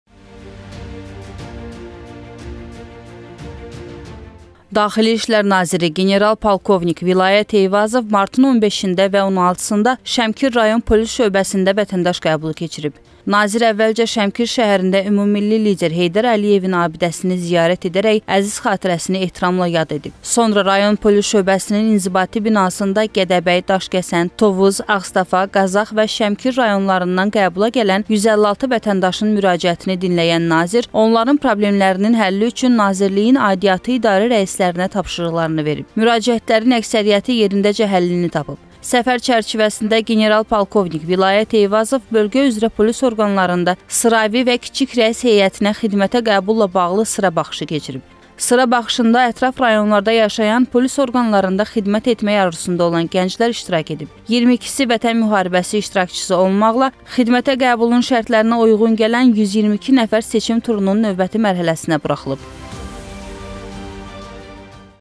Vilayət Eyvazov Şəmkirdə vətəndaş qəbulu keçirdi - BAKU TV
Daxili işlər naziri general-polkovnik Vilayət Eyvazov martın 15-də və 16-da Şəmkir Rayon Polis Şöbəsində vətəndaş qəbulu keçirib.
Nazir əvvəlcə Şəmkir şəhərində ümummilli lider Heydər Əliyevin abidəsini ziyarət edərək əziz xatirəsini ehtiramla yad edib. Sonra Rayon Polis Şöbəsinin inzibati binasında Gədəbəy, Daşkəsən, Tovuz, Ağstafa, Qazax və Şəmkir rayonlarından qəbula gələn 156 vətəndaşın müraciətini dinləyən nazir onların problemlərinin həlli üçün Nazirliyin aidiyyəti idarə rəislərinə tapşırıqlar verib. Müraciətlərin əksəriyyəti yerindəcə həllini  tapıb.
Səfər çərçivəsində general-polkovnik Vilayət Eyvazov bölgə üzrə polis orqanlarında sıravi və kiçik rəis heyətinə xidmətə qəbulla bağlı sıra baxışı keçirib.
Sıra baxışında ətraf rayonlarda yaşayan, polis orqanlarında xidmət etmək arzusunda olan gənclər iştirak edib. 22-si Vətən müharibəsi iştirakçısı olmaqla xidmətə qəbulun şərtlərinə uyğun gələn 122 nəfər seçim turunun növbəti mərhələsinə buraxılıb.

Vilayət Eyvazov Şəmkirdə vətəndaş qəbulu keçirdi